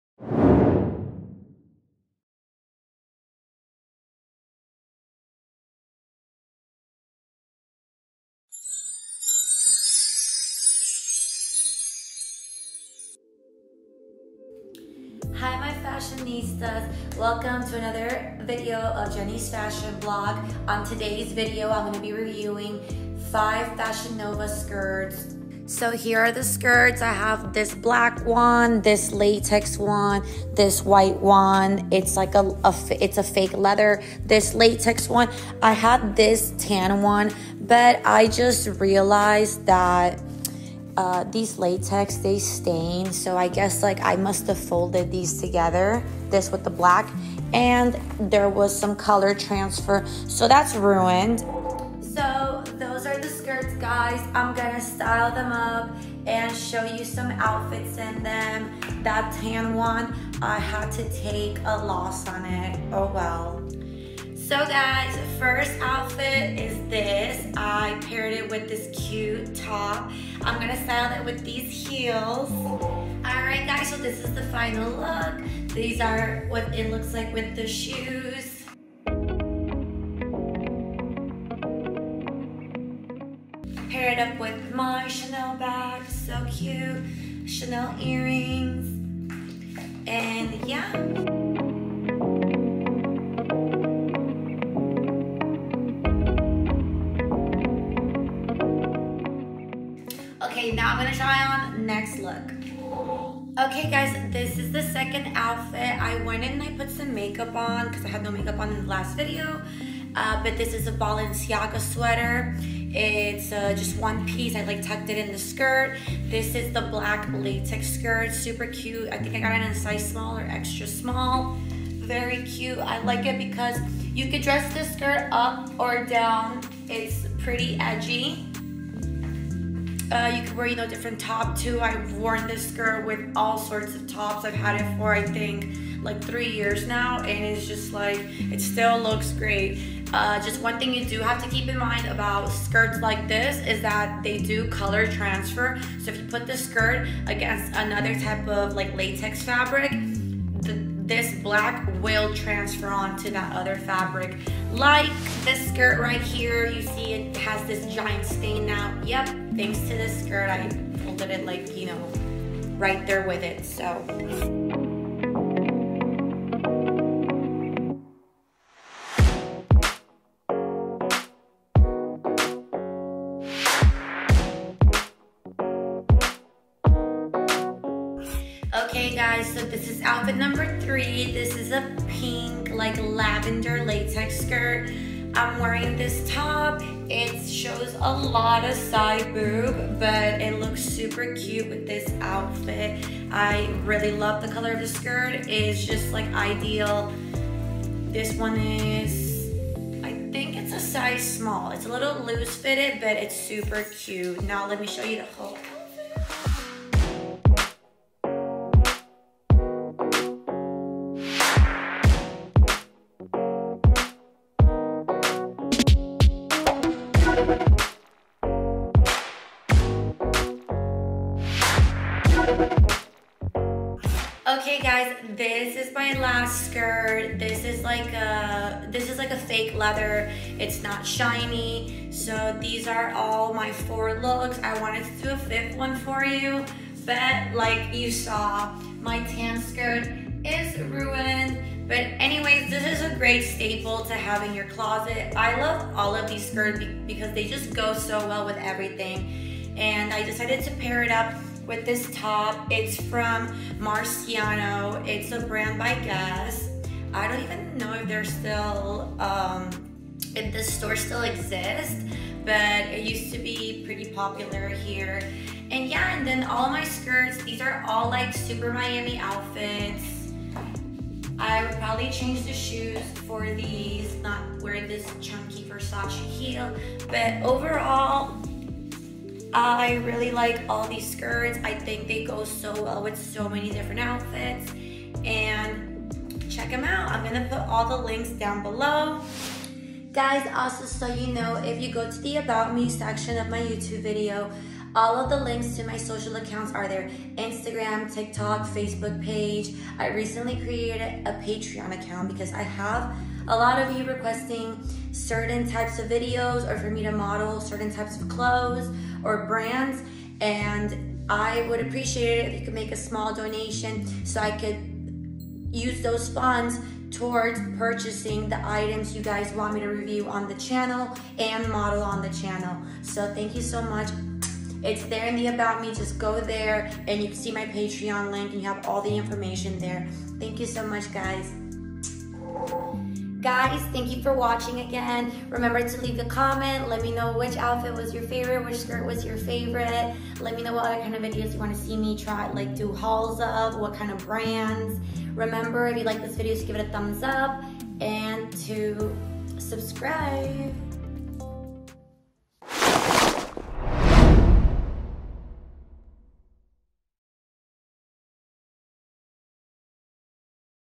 Try On Haul - I Buy and Try Fashion Nova Mini Skirts and Review Them For You
All skirts are from fashion nova the latex skirts
Are called boot camp skirt

Black size xs
Purple s
Tan s

The other faux leather skirts

Buy Me A Drink Skirt - Black and white size small